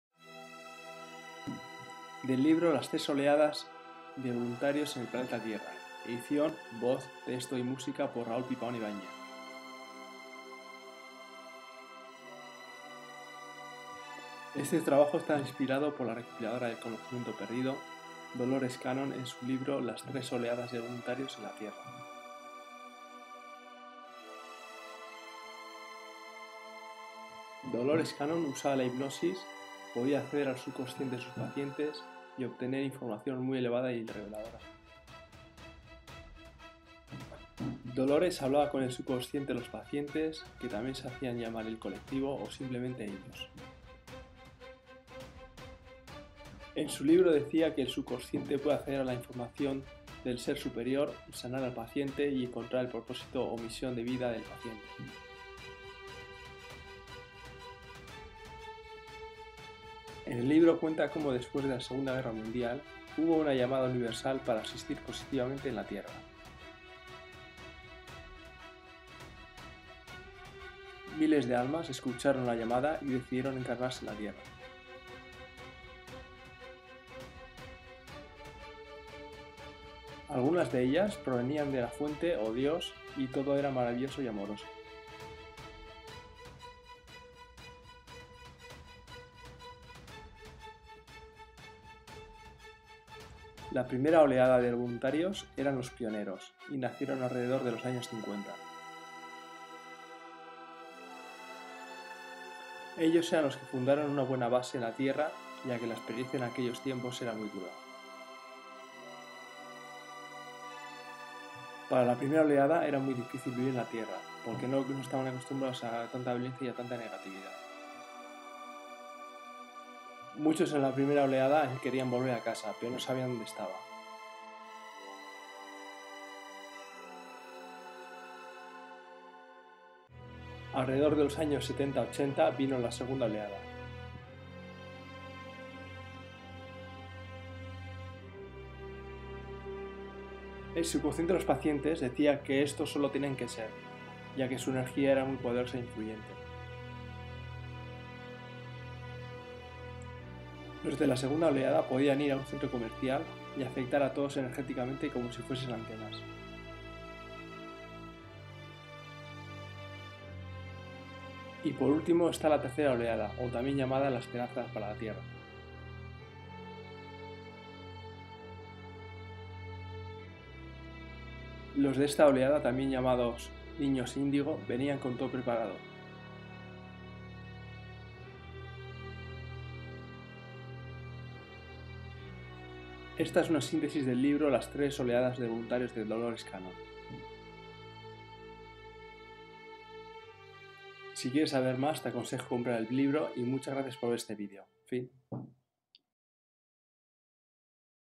DEL LIBRO LAS 3 OLEADAS DE VOLUNTARIOS EN LA TIERRA #20.- raúl pipaon ibañez - goldenphi
Para comprar el libro puedes buscar en amazon el nombre Dolores Cannon. Aqui el texto en castellano:

Este trabajo está inspirado por la recopiladora del conocimiento perdido Dolores Cannon en su libro Las 3 oleadas de voluntarios en la Tierra. Dolores Cannon usaba la hipnosis podía acceder al subconsciente de sus pacientes y obtener información muy elevada y reveladora. Dolores hablaba con el subconsciente de los pacientes, también el colectivo o simplemente “ellos” que también se hacían llamar. En su libro decía que el subconsciente puede acceder a información del ser superior, sanar al paciente y encontrar el proposito/misión de la vida del paciente. En el libro cuenta como después de la segunda guerra mundial hubo una llamada universal para asistir positivamente en la Tierra. Miles de Almas escucharon la llamada y decidieron encarnarse en la Tierra. Algunas de ellas provenian de la Fuente o Dios o su casa y que todo era maravilloso y amoroso. La primera ola de voluntarios eran los pioneros y nacieron alrededor de los años 50. Ellos eran los que fundaron la base en la Tierra ya que la experiencia en aquellos tiempos era muy dura. Para la primera ola era muy dificil vivir en la Tierra porque no estaban acostumbradas a tanta violencia y tanta negatividad. Muchos en la primera ola querían volver a casa pero no sabían donde estaba. Alrededor de los años 70-80 vinieron la segunda ola. El subconsciente de los pacientes decía que estos solo tenían que ser ya que su energía era muy poderosa e influyente. Los de la segunda ola podían ir a centro comercial y afectar a todos energeticamente como si fuesen antenas. Y por último, esta la tercera ola o tambien llamada la esperanza para la Tierra. En esta ola los de esta ola también llamados niños indigo venían con todo ya preparado. Este es una sintesis del libro las 3 olas de voluntarios de Dolores Cannon.